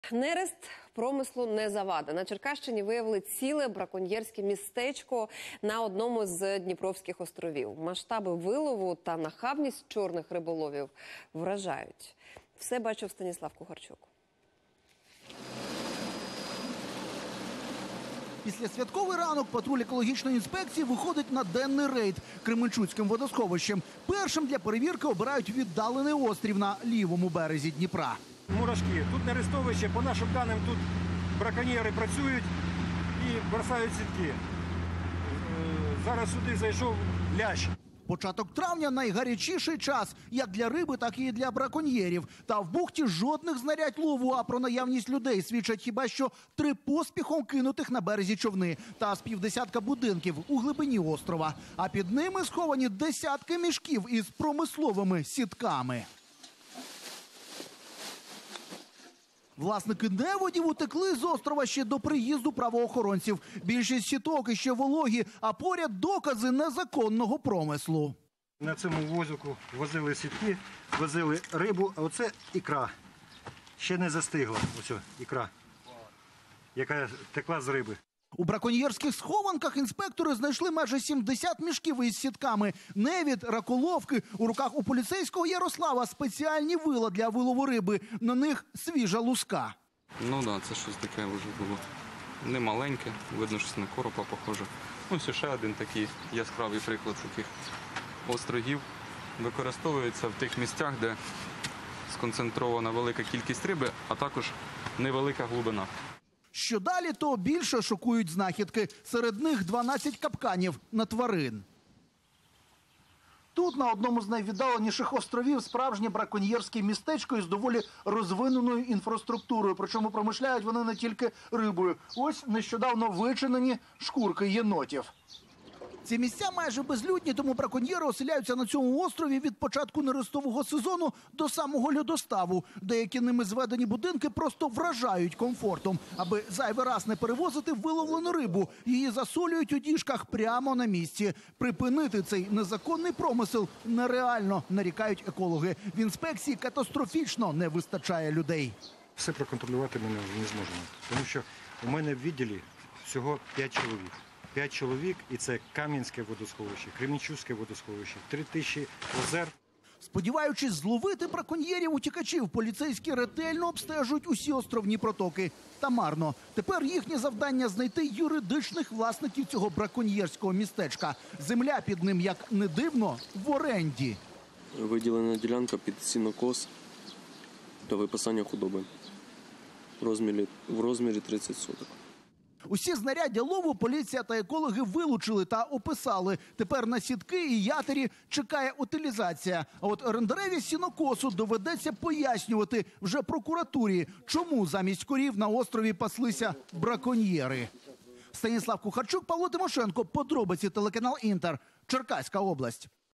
На Черкащині виявили браконьєрське містечко
Післясвятковий ранок. Патруль екологічної інспекції виходить на денний рейд Кременчуцьким водосховищем. Першим для перевірки обирають віддалений острів на лівому березі Дніпра